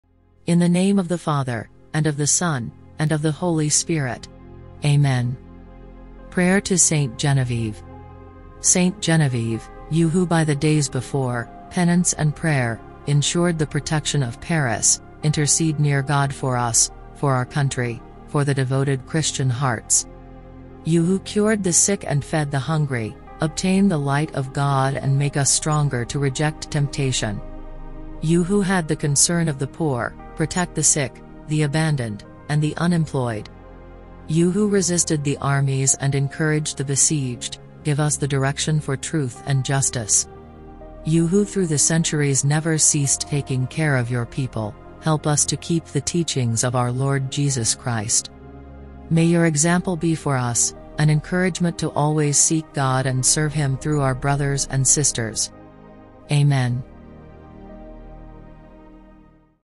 Prayer With Saint Genevieve
Genevieve is the patroness saint of Paris in the Catholic and Orthodox traditions. Her feast is on 3 January. Genevieve was born in Nanterre and moved to Paris after encountering Germanus of Auxerre and Lupus of Troyes and dedicated herself to a Christian life.